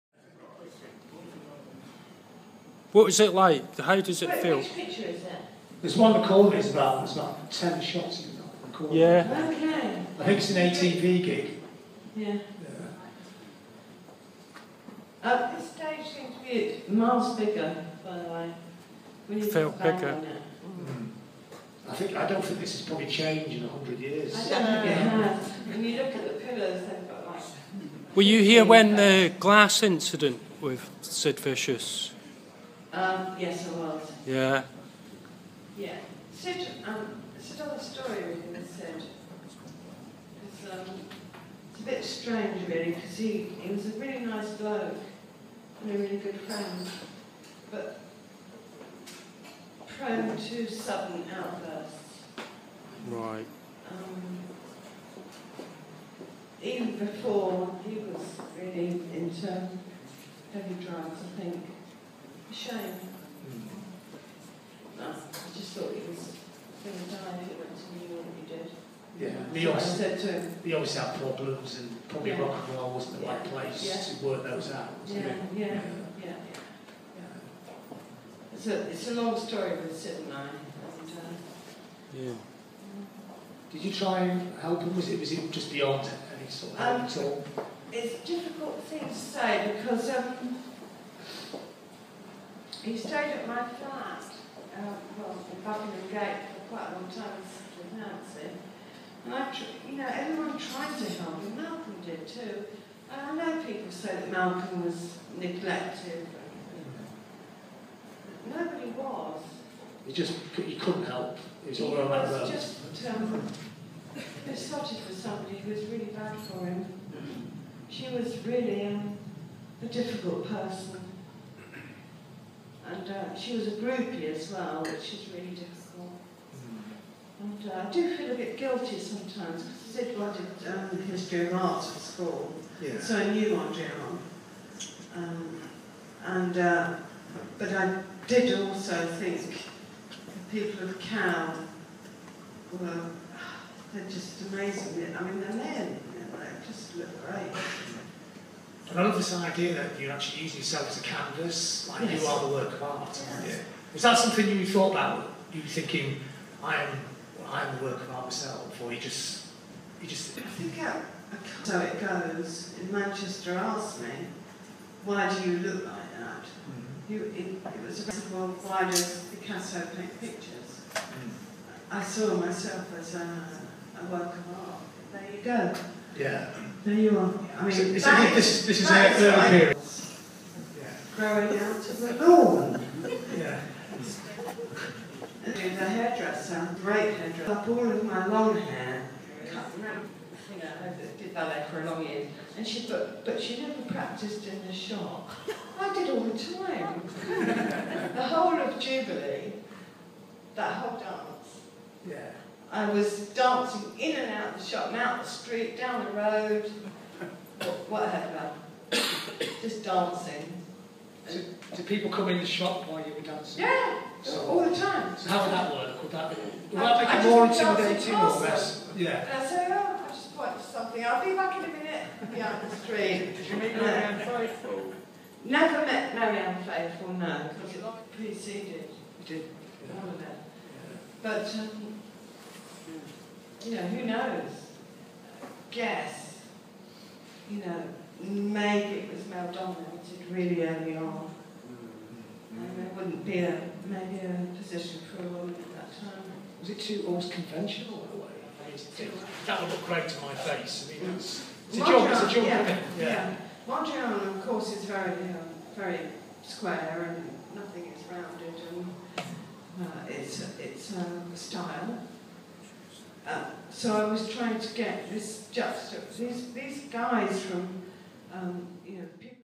Jordan (original punk at SeX insights on Sid Vicious) live at the 100 Club
Jordan Mooney in conversation with John Robb at the 100 Club. Original London punk story actress in The Great Rock n Roll Swindle & Jubilee. Malcolm McLaren Vivienne Westwood shop SeX. Talks about Sid Vicious